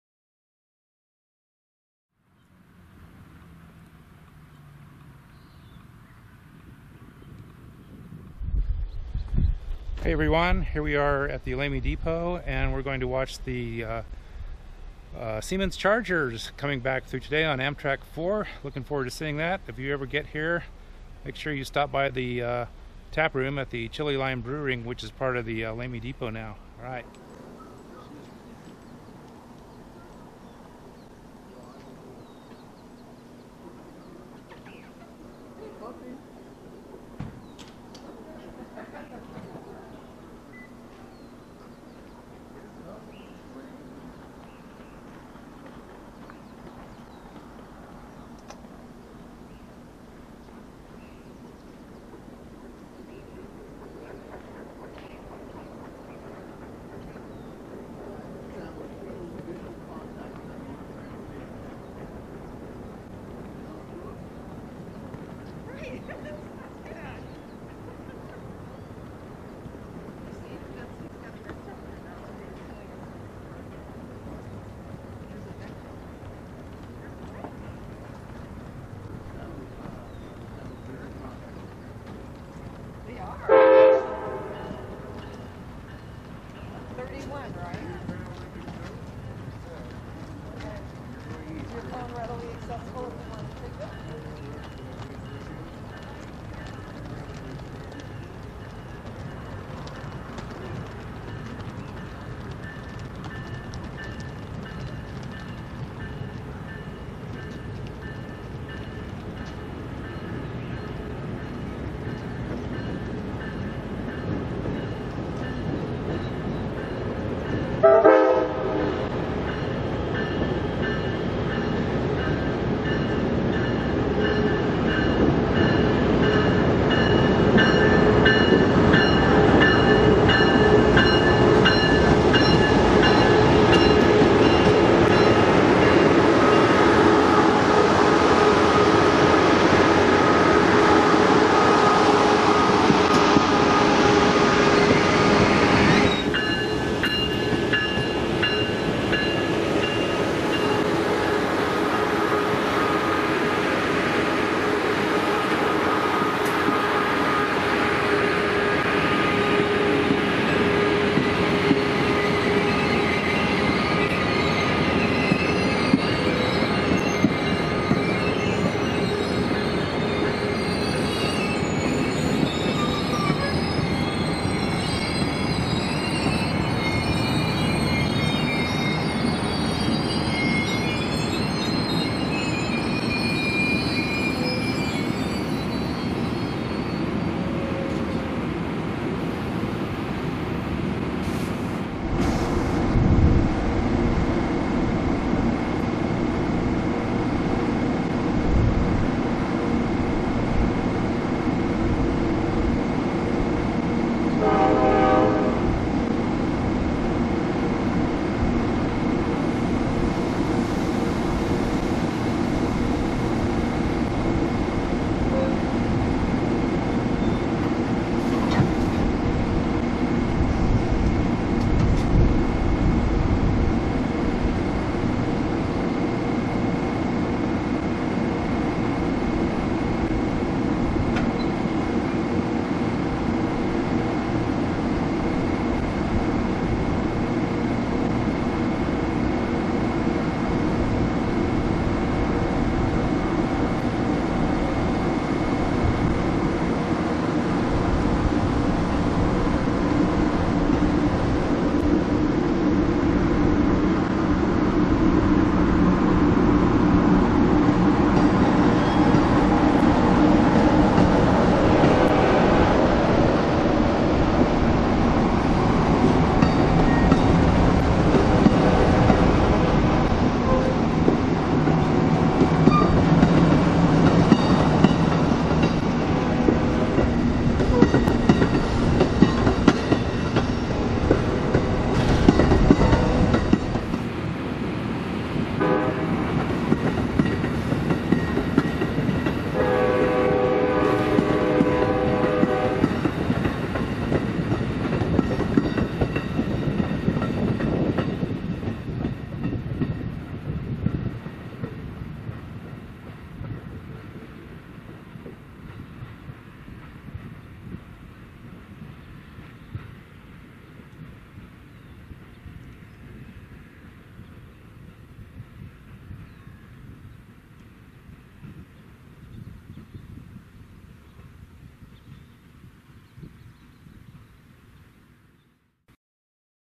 Siemens Chargers Return on the Southwest Chief!
The Siemens Chargers return to New Mexico on the eastbound Southwest Chief.  Seen in Lamy on August 1, 2019, these engines are being tested providing power for the first time on Amtrak 4.